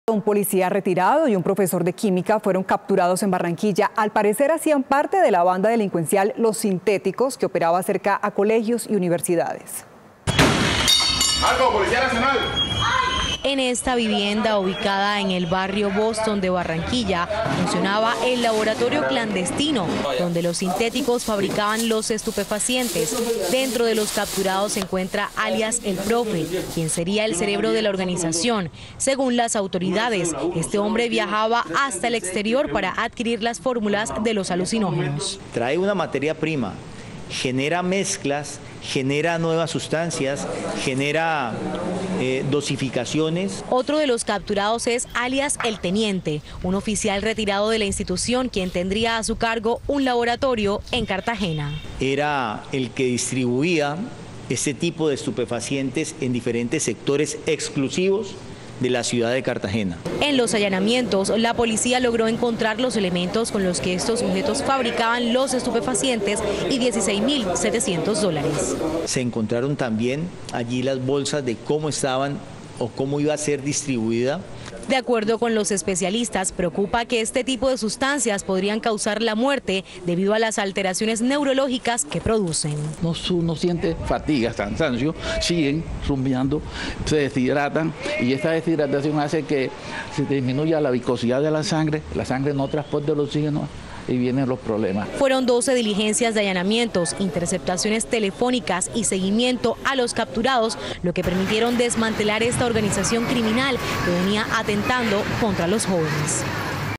Un expolicía y un profesor de química formaban parte de la banda ‘Los sintéticos’ | Noticias Caracol
Delinquían cerca de colegios y universidades en Barranquilla. Los capturaron tras seguimientos e interceptaciones telefónicas.